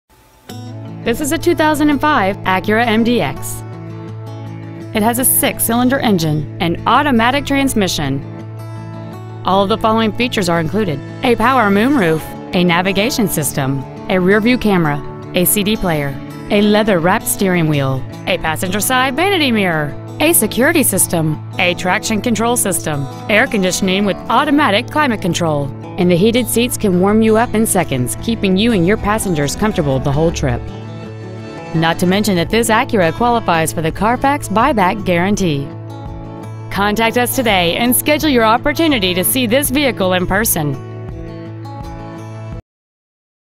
2005 Acura MDX San Jose CA 95129
Year : 2005

 Make : Acura

 Model : MDX

 Engine : 3.5L V-6 cyl

 Trans . : Automatic

 Exterior : Black

 Miles : 96,169

 Interior : Beige

 4590 Stevens Creek Boulevard

 Pre-Owned 2005 Acura MDX San Jose CA